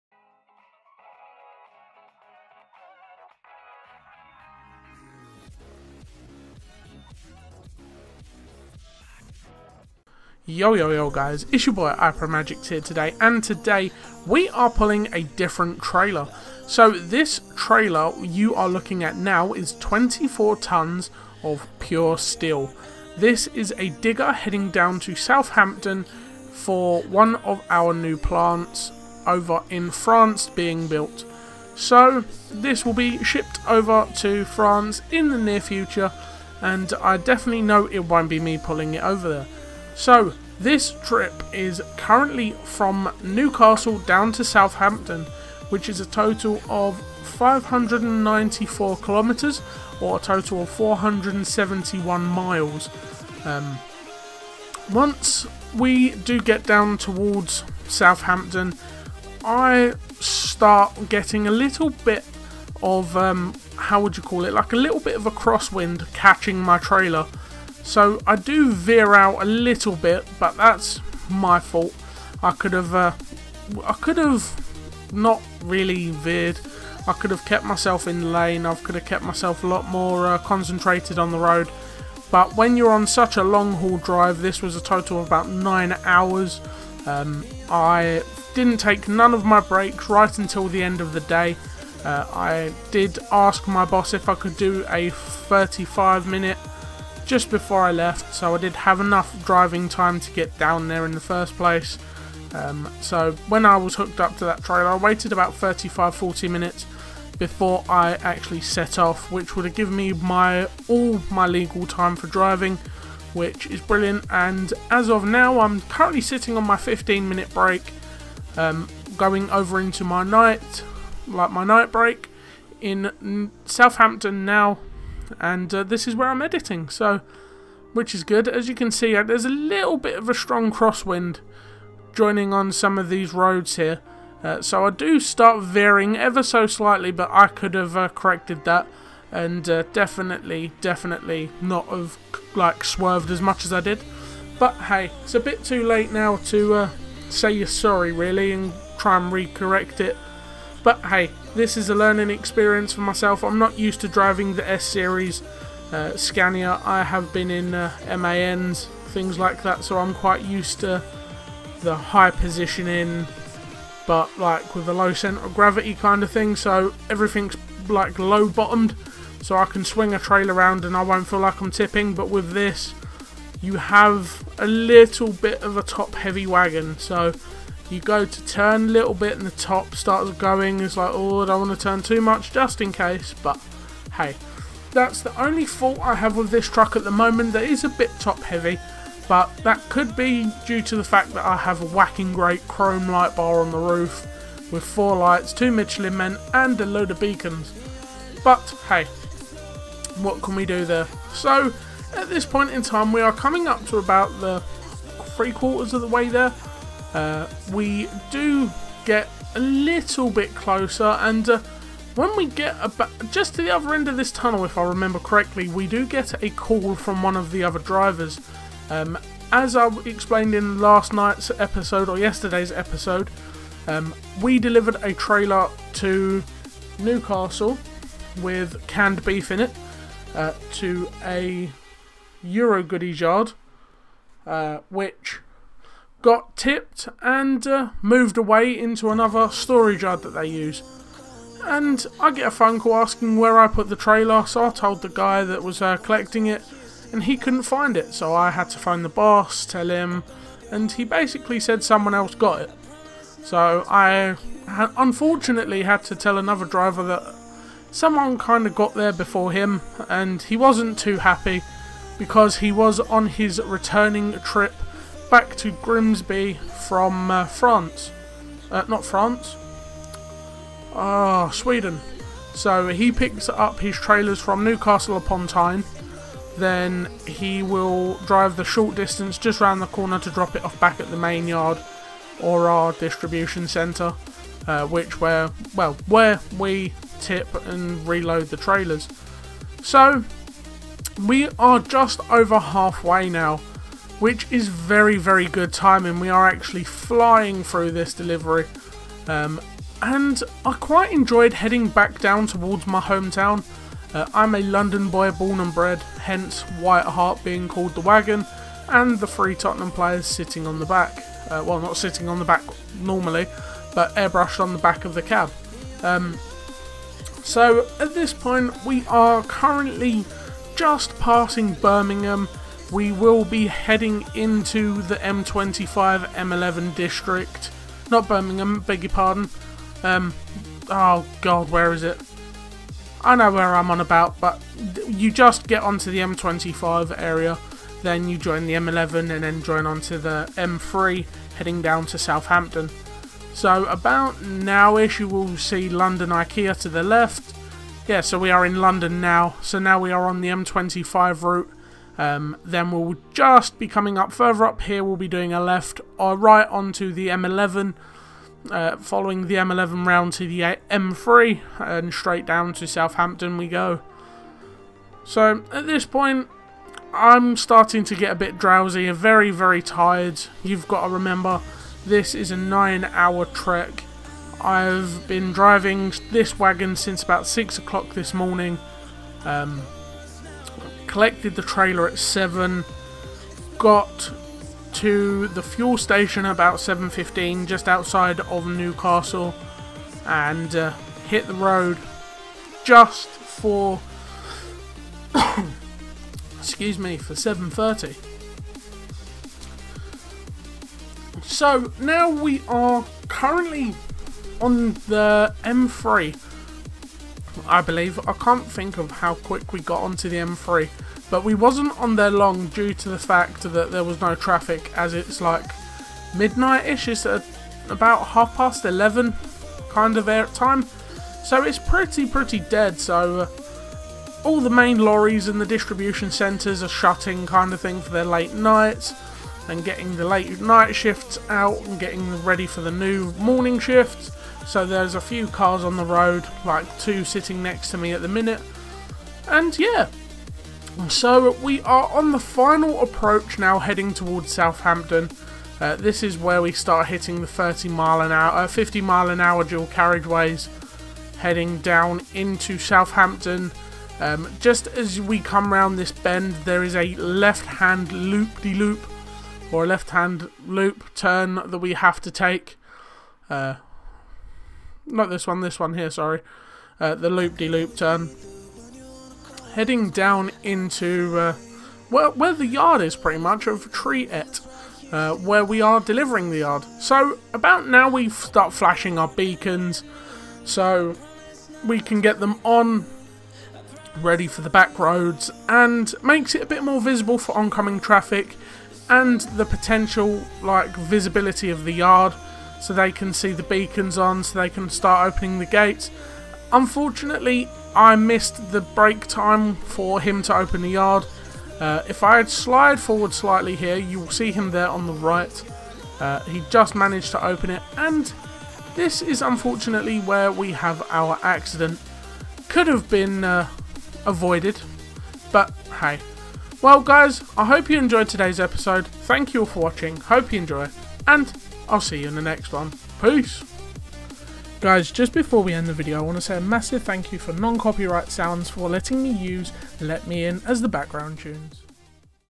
I HAD A ACCIDENT! (RT1MM EP11)
Time For Road To One Million Miles Again!
Today We Are Pulling 24 ton Digger Heading Down To Southampton Which Will Be Heading to France Soon!
In My New Scania S Series Also Known As WhiteHart (WHL)

My Mod List:
Coming Soon!